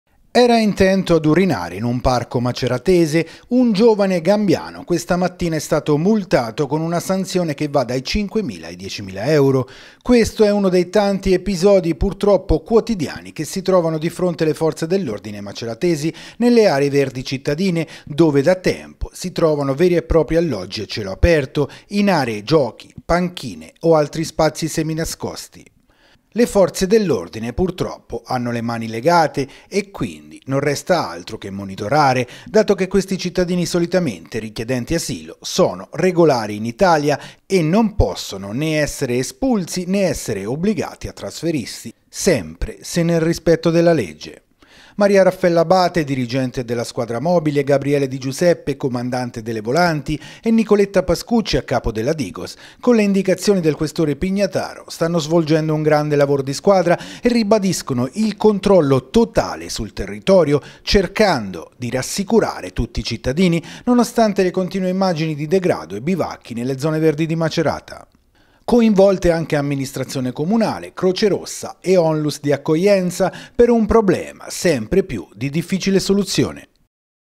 Polizia di Stato, monitoraggio continuo nelle aree a rischio di Macerata
Macerata. 25 settembre 2018. Continuo monitoraggio della Polizia di Stato nelle aree a rischio della città...